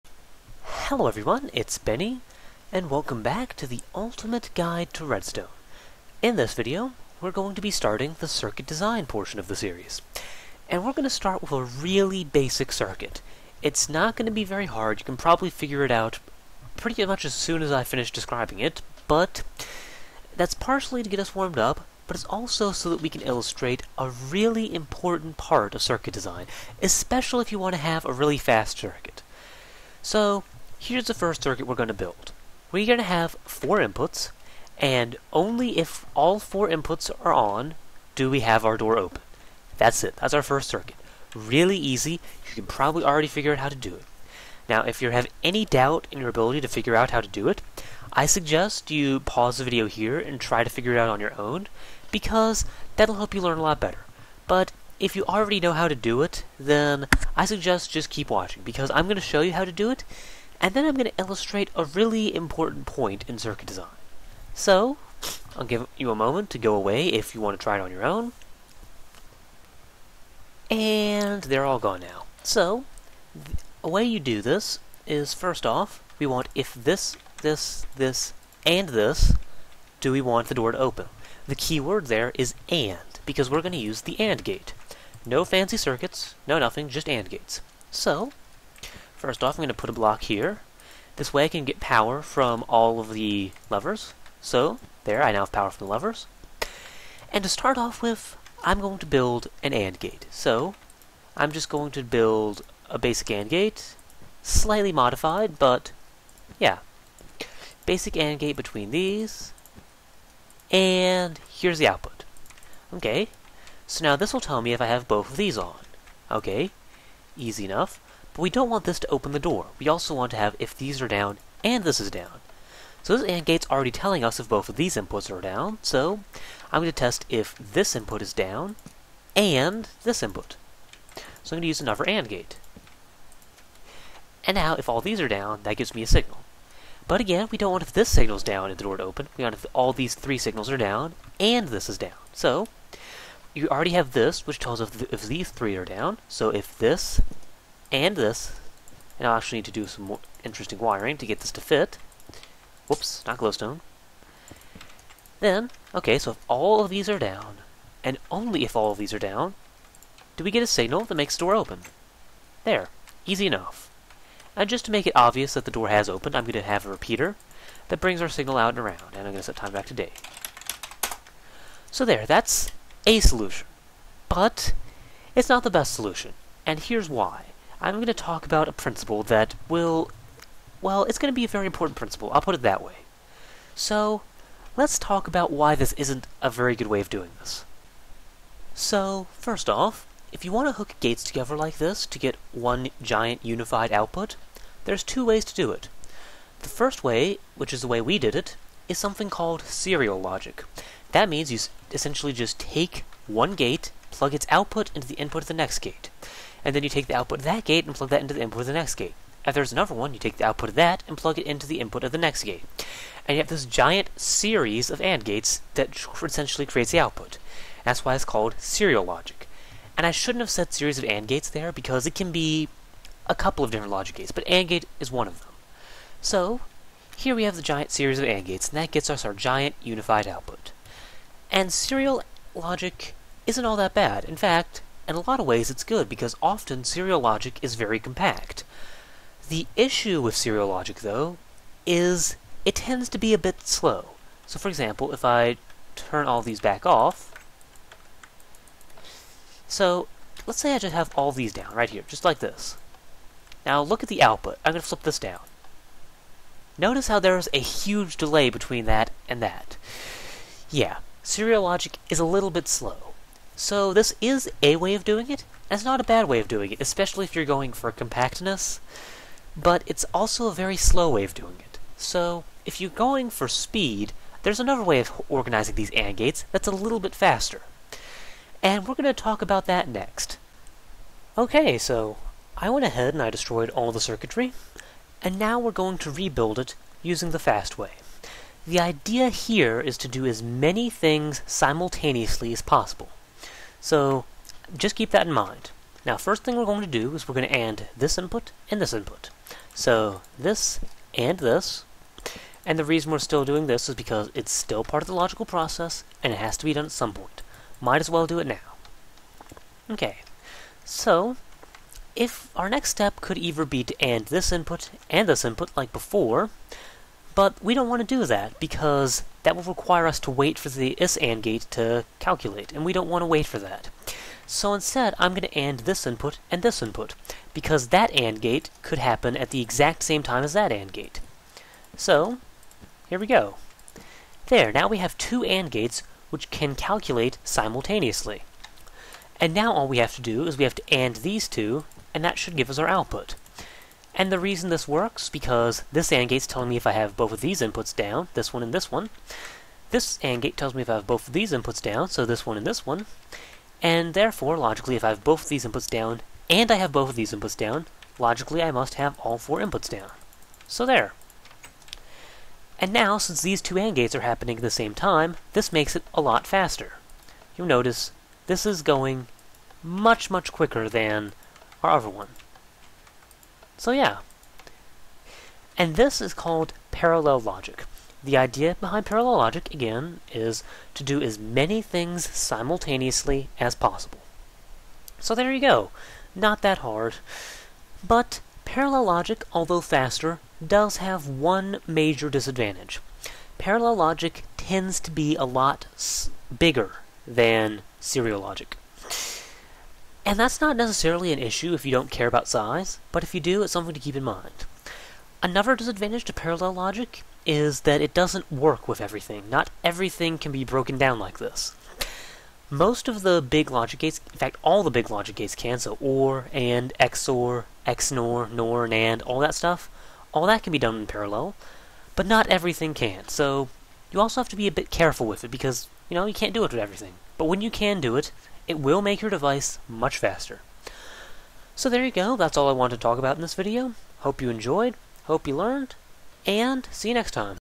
Redstone Circuits 6: Serial Logic vs. Parallel Logic
This video talks about the difference between serial and parallel logic, and their advantages and disadvantages.

After this video I'll be moving on to the next section of the series. However, this section isn't done. I've just covered all I want to here and I think that starting the next section sooner will be more beneficial.